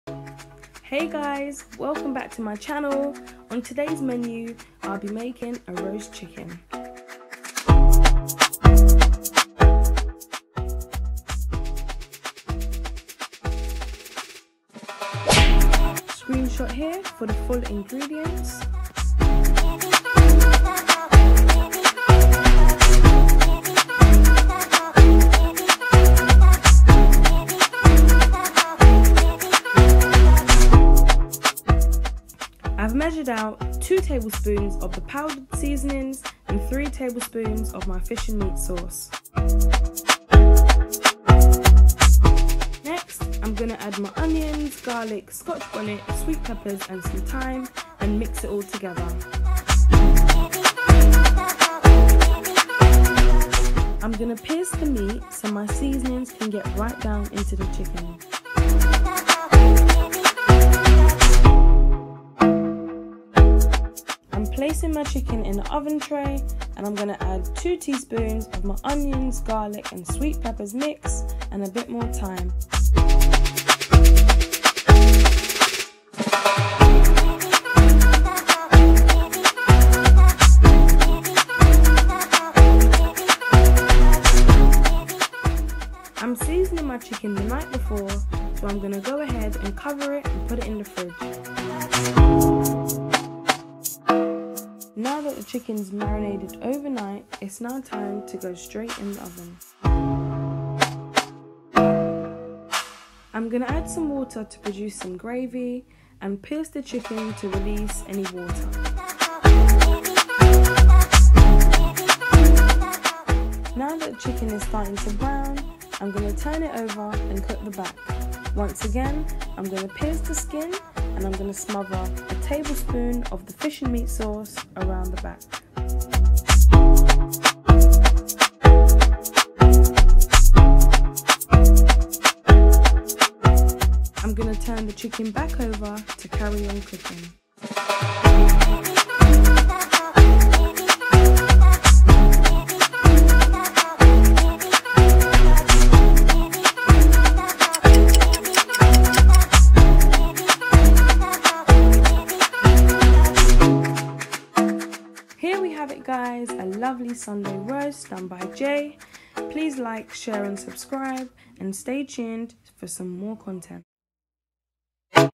JAYS FAVOURITE CUISINES | SUNDAY ROAST CHICKEN
Lovely Sunday Roast Chicken Tutorial ! I hope you love it and if you try it please share a photo, comment or let me know! X

Ingredients:

-  Whole Chicken
- All Purpose Seasoning (any)
- Chicken seasoning  (any)
- Hot & Spicy Seasoning (any)
- Black Pepper (any)
- Fish & Meat Sauce
- Garlic
- Onions
- A Sweet Pepper
- Thyme
- Scotch Bonnet